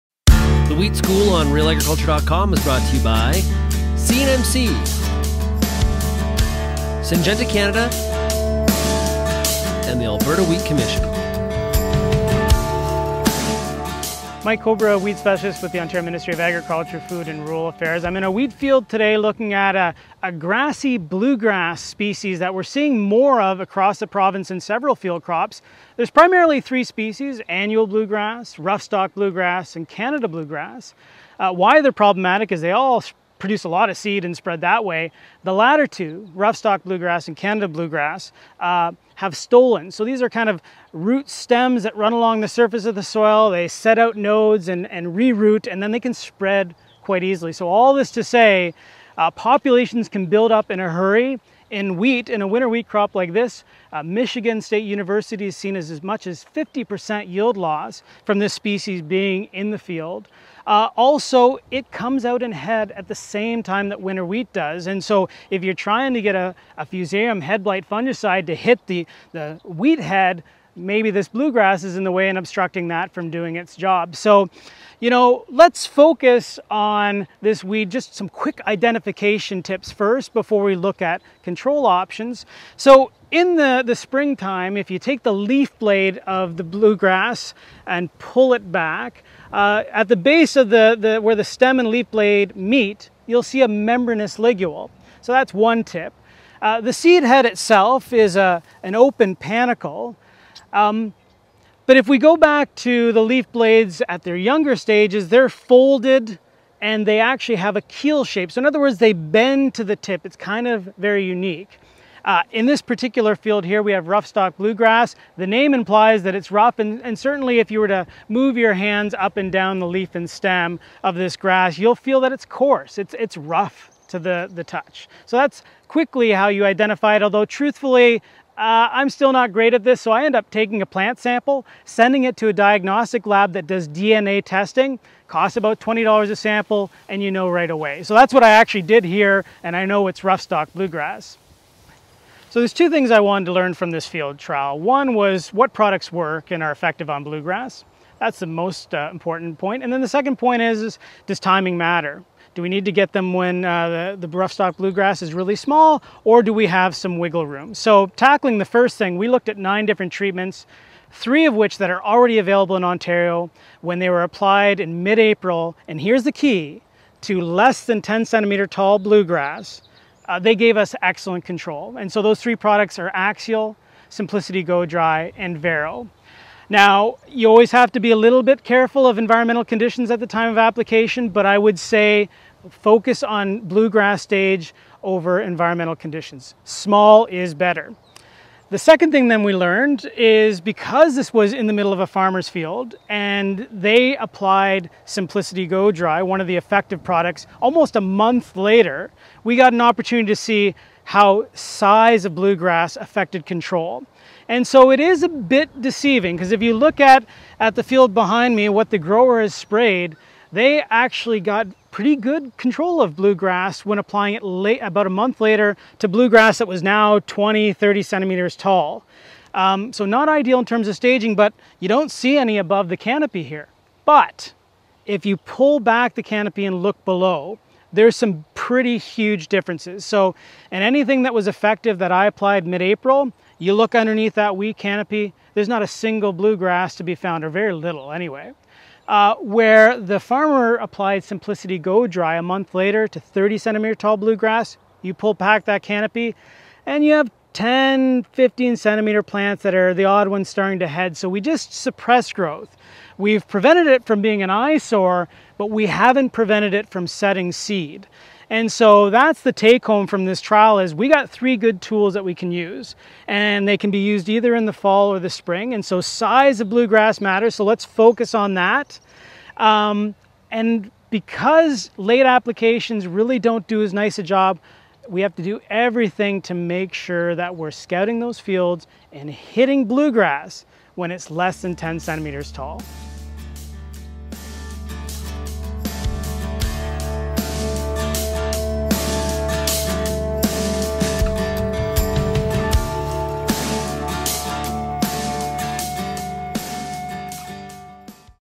Wheat School: Getting the better of Bluegrass
Grain growers are reporting that bluegrass is becoming a growing weedy issue in many Ontario field crops. There are three types of weedy bluegrass species that are being found: annual bluegrass, rough-stalk bluegrass, and Canada bluegrass.

As Mike Cowbrough, OMAFRA weed specialist explains in this Wheat School episode, the reason bluegrass is such a problem is because all three species are prolific seed producers and spreader. The latter two species, rough-stalk and Canada bluegrass, spread by stolons — root stems that travel along the surface of the soil, send out nodes, and root themselves in.